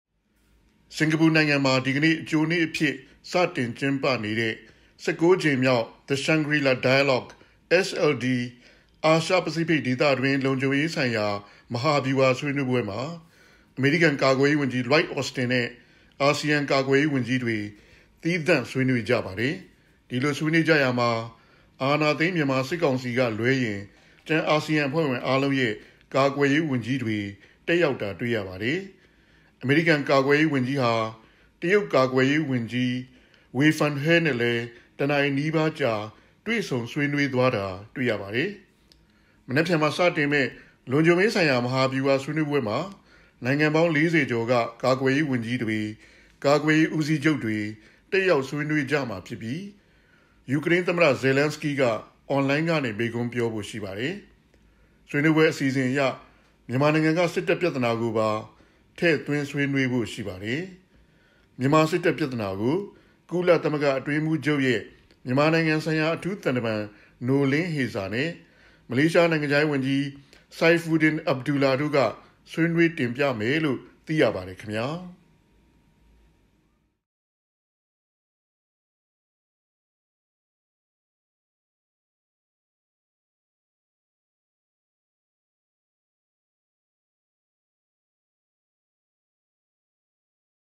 အမေရိကန်-အာဆီယံကာကွယ်ရေးဝန်ကြီးများဆွေးနွေးပွဲ မြန်မာ စကစ တက်ခွင့်မရ
၁၀/၆/၂၀၂၂
စင်ကာပူမှာ မနက်ဖန်စတင်ကျင်းပမဲ့ အင်ဒိုပစိဖိတ် လုံခြုံရေးဆိုင်ရာထိပ်သီးဆွေးနွေးပွဲမှာ ယူကရိန်း၊ တောင်တရုတ်ပင်လယ်နှင့် မြန်မာ့အရေးအဓိကဆွေးနွေးကြမှာဖြစ်သလို ဒီကနေ့ အကြိုကျင်းပတဲ့ အမေရိကန် အာဆီယံကာကွယ်ရေးဝန်ကြီးများအစည်းအဝေးမှာလည်း အာဏာသိမ်းစကစတက်ခွင့်မရခဲ့ပါဘူး။